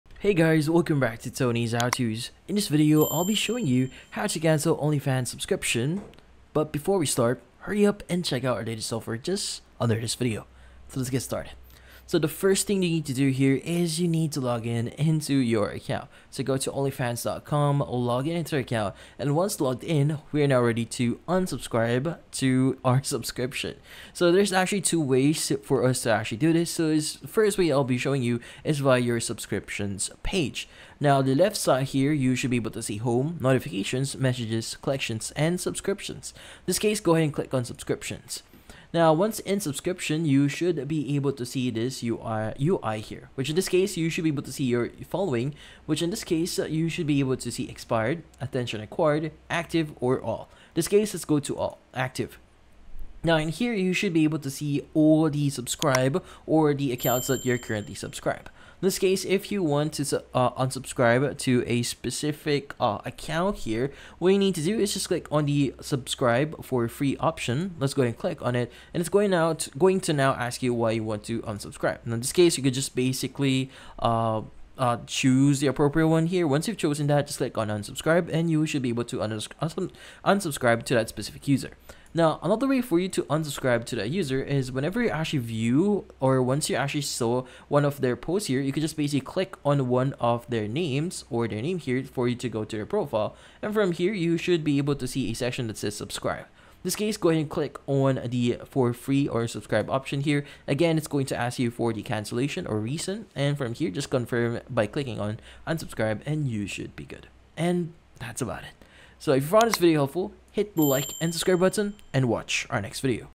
How to Cancel Onlyfans Subscription (Quick Step by Step Tutorial, 2024)
In this video, I show you How to Cancel Onlyfans Subscription (Quick Step by Step Tutorial) in 2024. The quick and easy tutorial how to delete onlyfans subscription. Do you want to know step by step how to stop onlyfans subscription in 2024? Yes? Great!